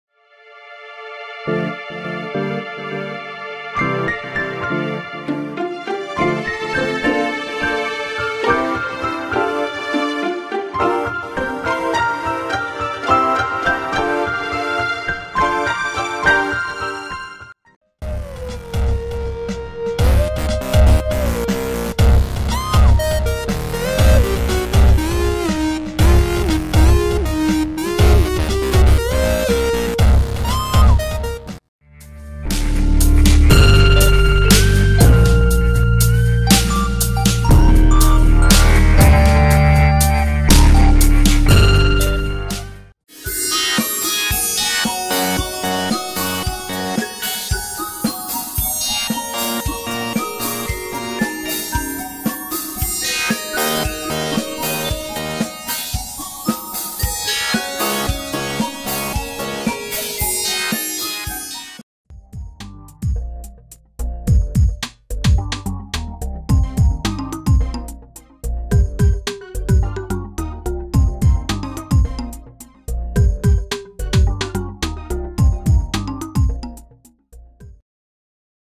GreenPillowGames Music Reel / Peter Wight
This was created as a social media promotion for the Purplegg metaverse.

All music was created by Peter Wight